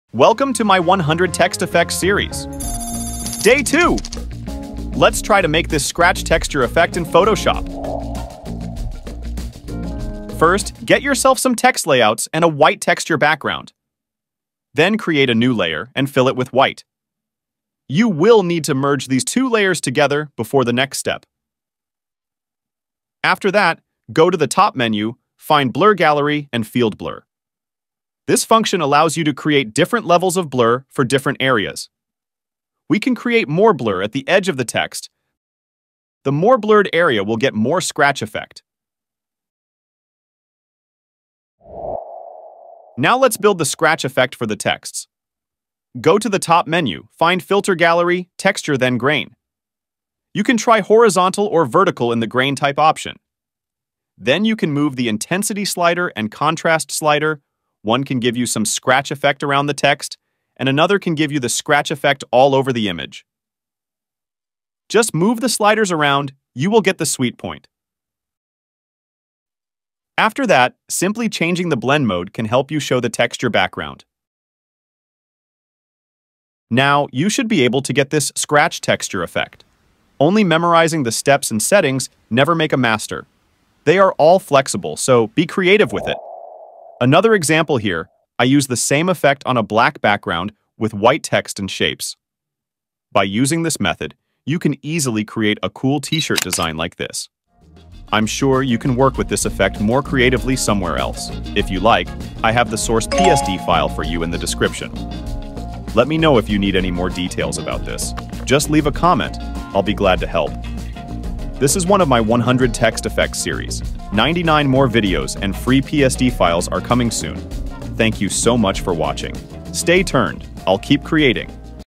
Scratch Text Effect in Photoshop + Free PSD File | Blur & Grain Filter [100 Text Effects]
Day Two! Let's try to make this scratch texture text effect in photoshop.

0:00 Intro
0:28 Blur the Text
0:42 Build the Scratch Effect with Grain Filter
1:27 Scratch Effect for T-Shirt Design